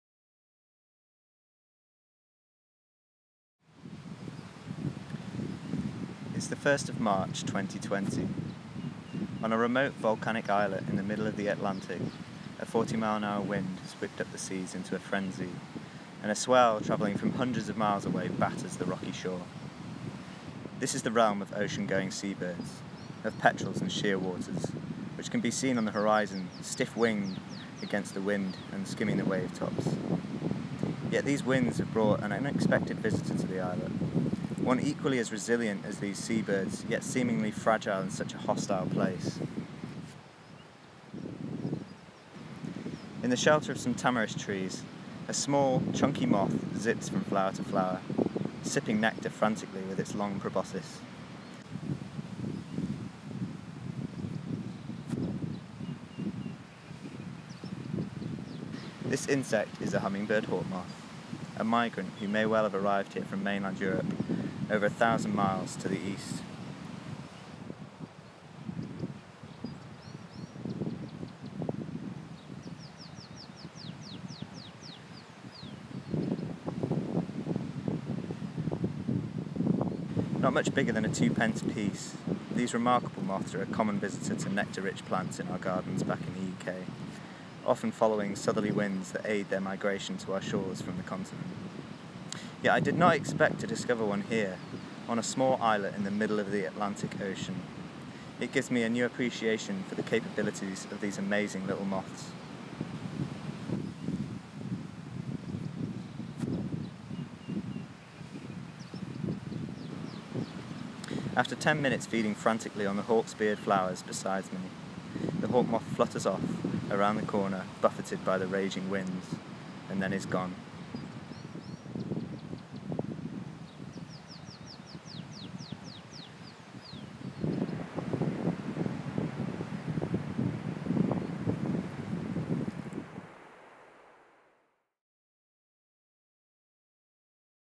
An unexpected visitor
A Hummingbird Hawkmoth pays a surprise visit to a remote offshore island in the middle of the Atlantic. Feeding on a small patch of hawksbeard flowers in a sheltered glade, it allows me to spend an amazing ten minutes taking slo-motion footage as it feeds frantically on the flowers' nectar. This film depicts its amazing feat in arriving here and the story of its discovery.